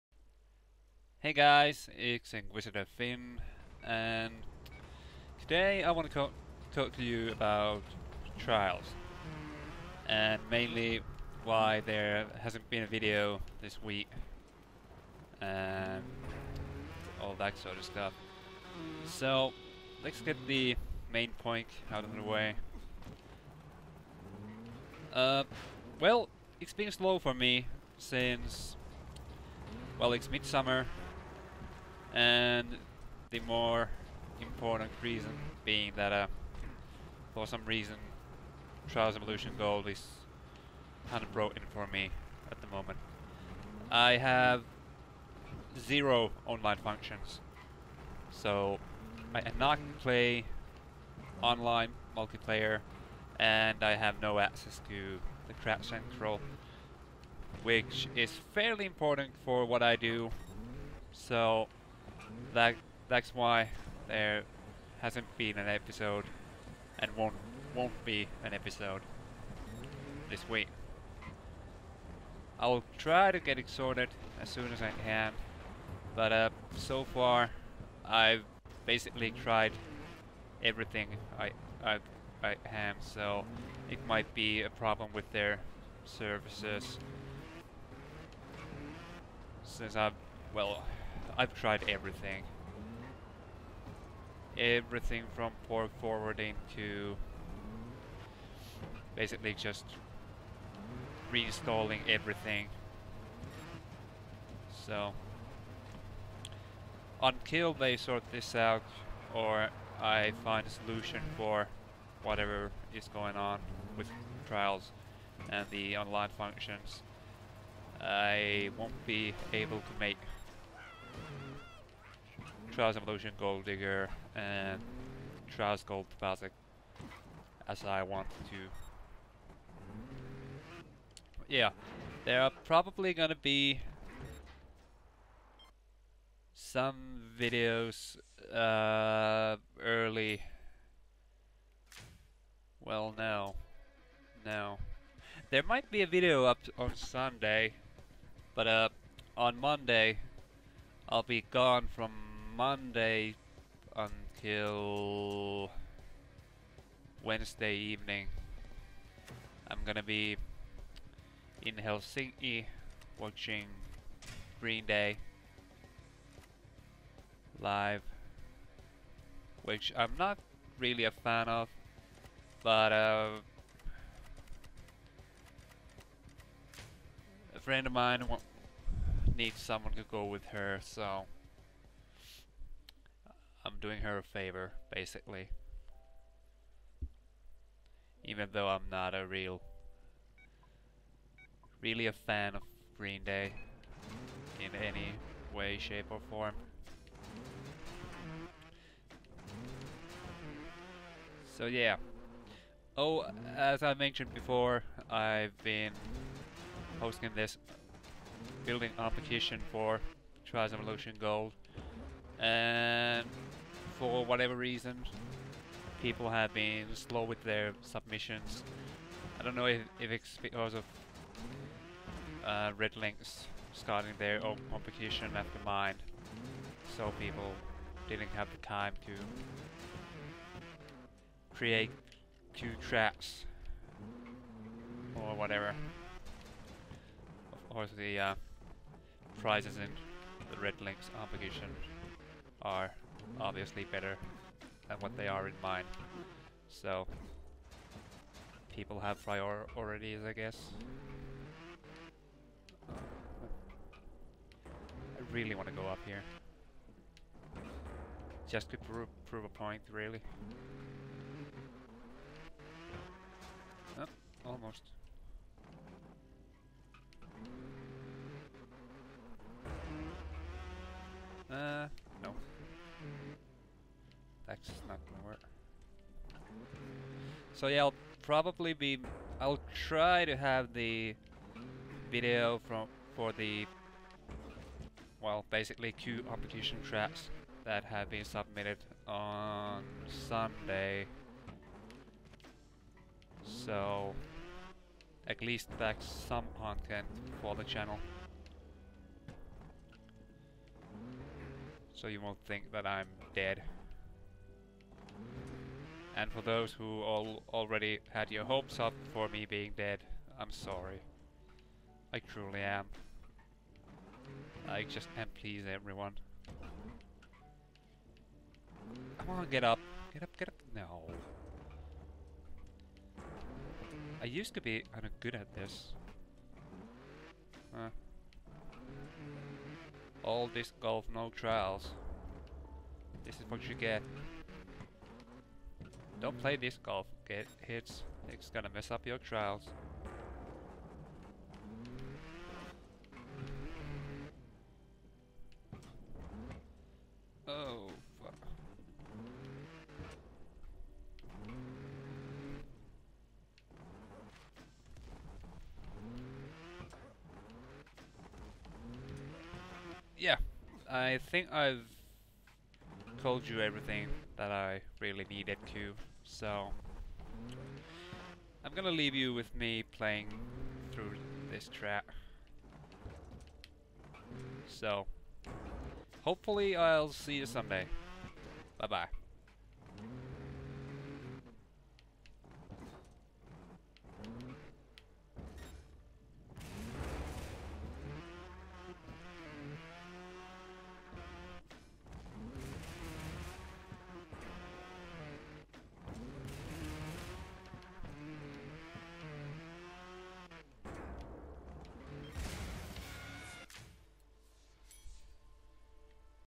Channel Announcement! [ OUTDATED ]
I got something to share with you guys regarding the lack of content on my channel.